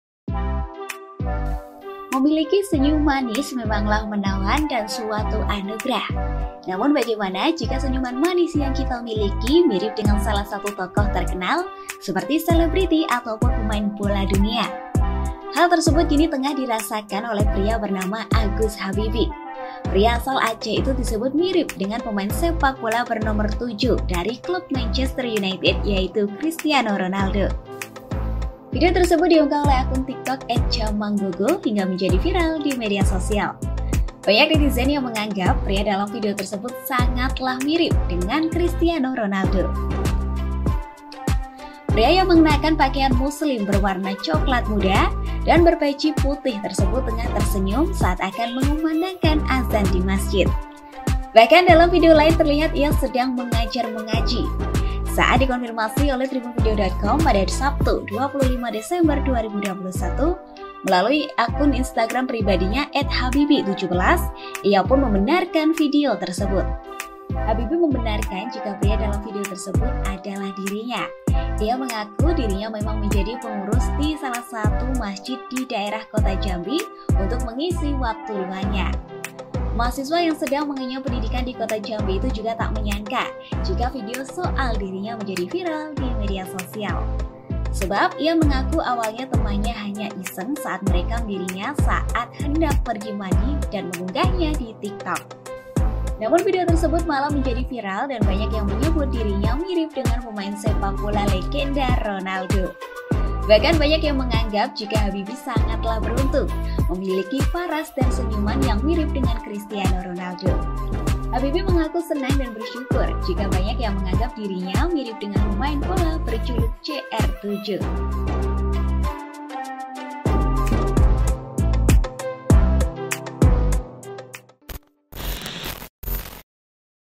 Viral Seorang Takmir Masjid Asal Aceh Disebut Miliki Senyum Mirip Christiano Ronaldo
Memiliki senyum manis memanglah menawan dan suatu anugerah.

Namun bagaimana jika senyuman manis yang kita miliki mirip dengan salah satu tokoh terkenal, seperti seleb ataupun pemain bola dunia?

Hal tersebut kini tengah dirasakan oleh pria bernama Agus Habibi (22).

Pria asal Aceh itu disebut mirip dengan pemain sepak bola bernomor 7 dari club Manchester United, Christiano Ronaldo.

Banyak netizen yang menganggap pria dalam video tersebut sangat mirip dengan Christiano Ronaldo.

"Pas senyum mirip banget weyy," tulis pemilik akun @Panjiekan.

Pria yang mengenakan pakaian muslim berwarna coklat muda dan berpeci putih tersebut tengah tersenyum saat akan mengumandangkan azan di masjid.

Bahkan dalam video lain terlihat ia sedang mengajar mengaji.

Saat dikonfirmasi oleh Tribun-video.com pada Sabtu (25/12/2021) melalui instagram pribadinya @hbbii17 ia membenarkan video tersebut.

Habibi membenarkan jika pria dalam tersebut adalah dirinya.

Ia mengaku dirinya memang menjadi pengurus di salah masjid di daerah Kota Jambi untuk mengisi waktu luangnya.

"Di jambi kak, Alhamdulillah bisa mengurangi beban hidup di jambi sambil ngisi waktu luang jadi marbot," terang Habibi.

Sebab ia mengaku awalnya temannya hanya iseng saat merekam dirinya saat hendak pergi mandi dan mengunggahnya di TikTok.

Namun video tersebut malah menjadi viral dan banyak yang menyebut dirinya mirip dengan pemain sepak bola legenda, Ronaldo.

"Awalnya diakun tiktok temen saya, dia iseng merekam saya pas lagi mau ngambil sabun buat mandi, malah viral," jelas Habibi.

Bahkan banyak yang menganggap Habibi sangat beruntung memiliki paras dan seyuman yang mirip dengan Christiano Ronaldo.

"Hoki banget muka mirip CR7, kereeen," tulis pemilik akun @alvyfernandaa.

Habibi mengaku senang dan bersyukur jika banyak yang menganggap dirinya mirip dengan pemain bola berjuluk CR7.

"Tentunya senang dong kak, karena bisa dimiripkan dengan pemain bola idola banyak orang," ungkapnya.

(Artikel ini telah tayang di Tribun-video.com/Fina)

Editor Video : Dimas Adi